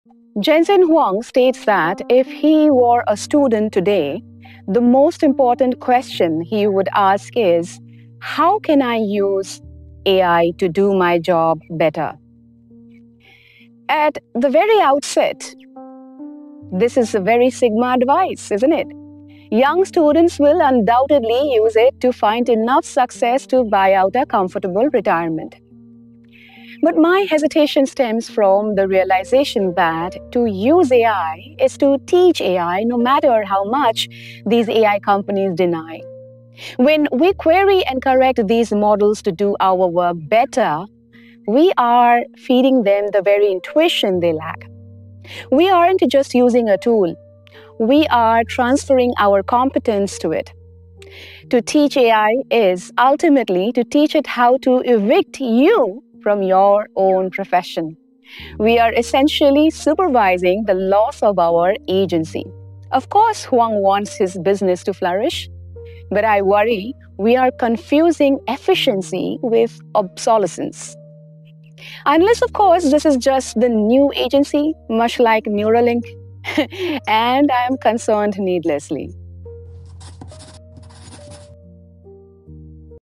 JENSEN HUANG & Obsolescence
Jensen Huang advises using AI to "do your job better." But are we just teaching it to replace us? 🤖📉

To use AI is to feed it our intuition. Are we supervising the loss of our own humanagency or finding new efficiency?